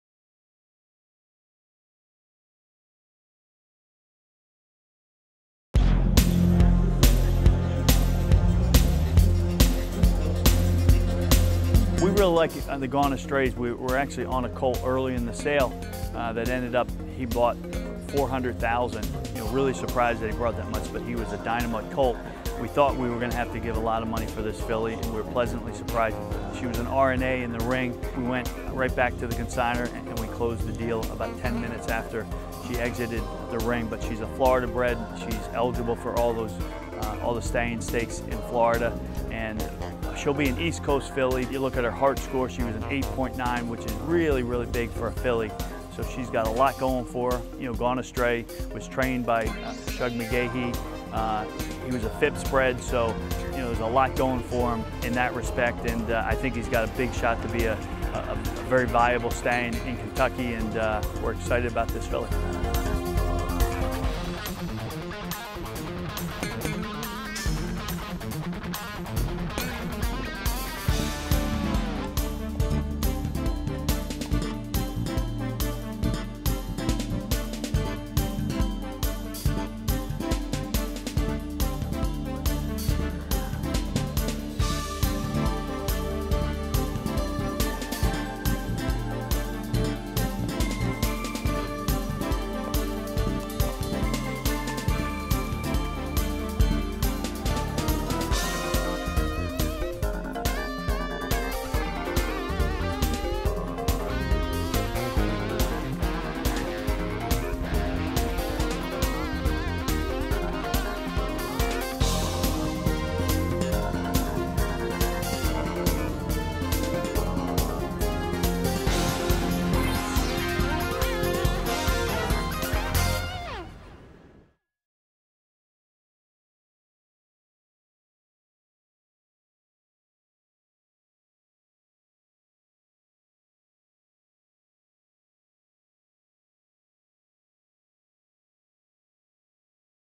Wayward Woman a Gone Astray Filly Horse Racing Syndicate OBS March 2016
See the Gone Astray filly named Wayward Woman at the 2016 OBS March sale. 2-year-old filly will race for one of the West Point Thoroughbreds horse racing syndicates. Racing partnerships enable individuals to own a high potential racehorse for a fraction of the cost and to enjoy all of the fun.